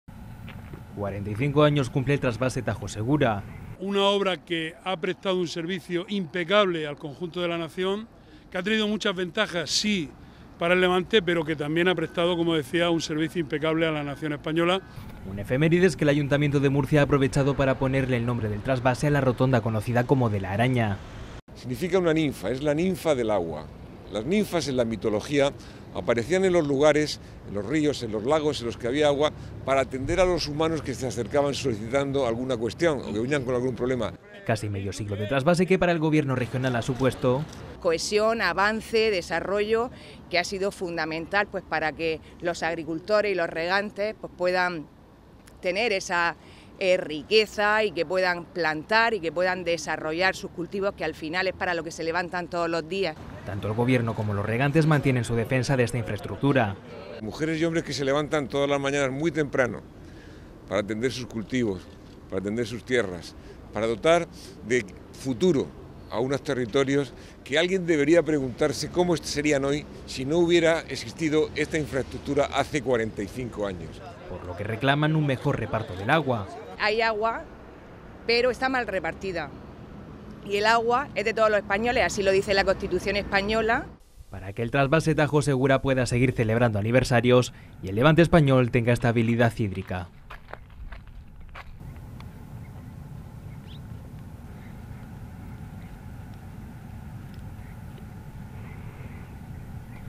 03/04/2024 Murcia inaugura una rotonda en homenaje al 45 aniversario del Trasvase
El ayuntamiento de Murcia ha aprovechado para renombrar la conocida como "rotonda de la araña", con el nombre de rotonda del Trasvase para homenajear a esta insfraestructura que han calificado de fundamental para la región.